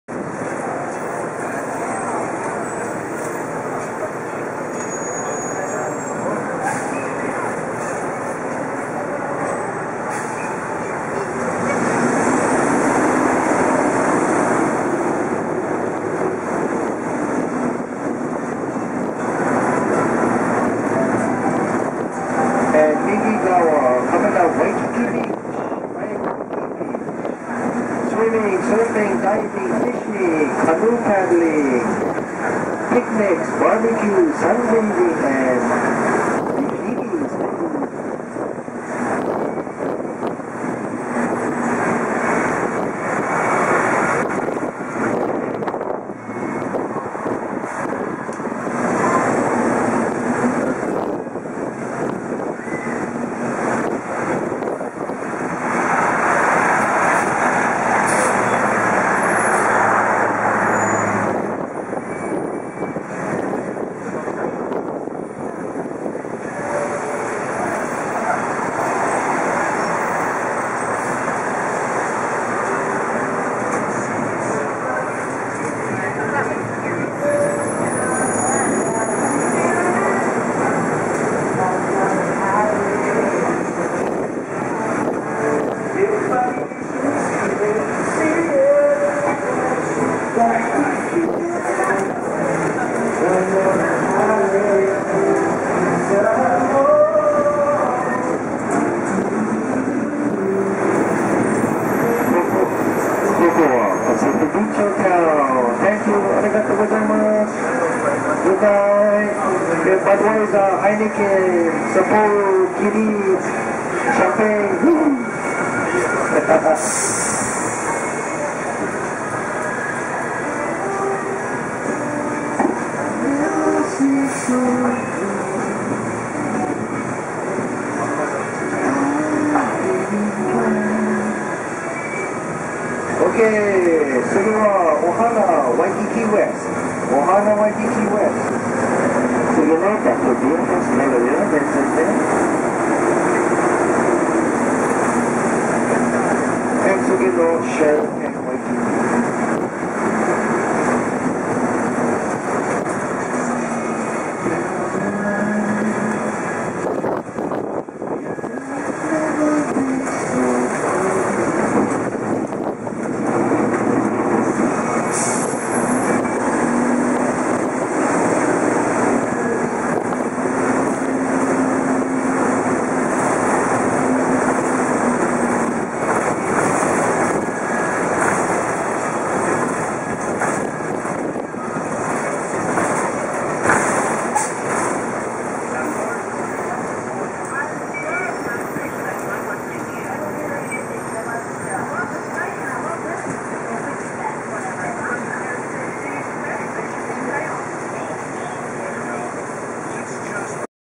2011年1月のハワイ旅行 走行するトローリー車内から見た10年前の夜のワイキキを少し
10年ともなると、変化もいろいろ。

映像に関していうと、10年以上前に購入したコンデジで撮影しているので、さすがに夜間の画質はボロボロ。現在のカメラの高感度画質とは雲泥の差。